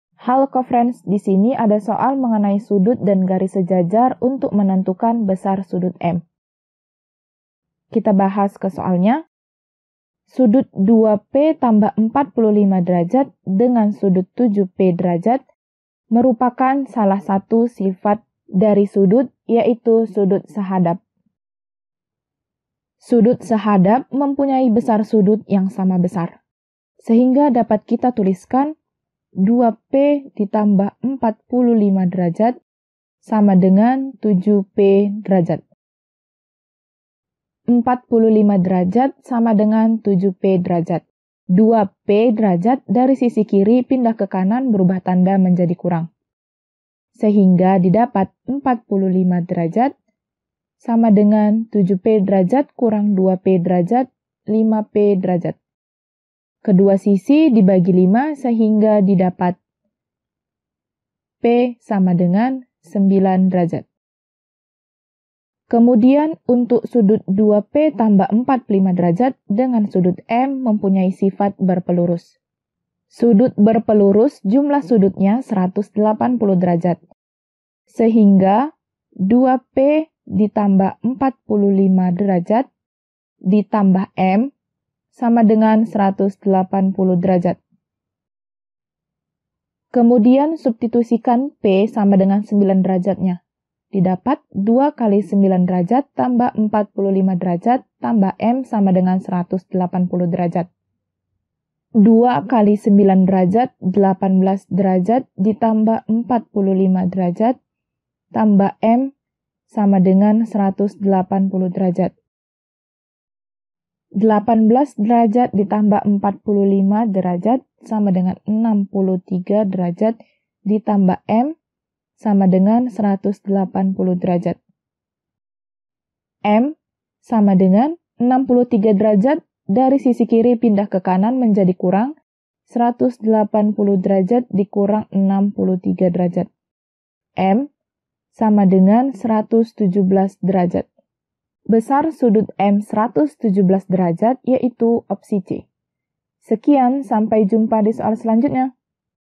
Besar sudut adalah m derajat: 8 A. 90 C. 1170 B. 630 D. 1330 (2p 45)9 7po m k
Topik atau Materi: Hubungan Antarsudut - SUDUT DAN GARIS SEJAJAR - GEOMETRI - Matematika Kelas 7 SMP - K-13 revisi

00:00 : Bedah Soal
01:00 : Konsep, rumus dan pengertian Hubungan Antarsudut
01:18 : Langkah penyelesaian soal
01:29 : Jawaban akhir
01:59 : Penutup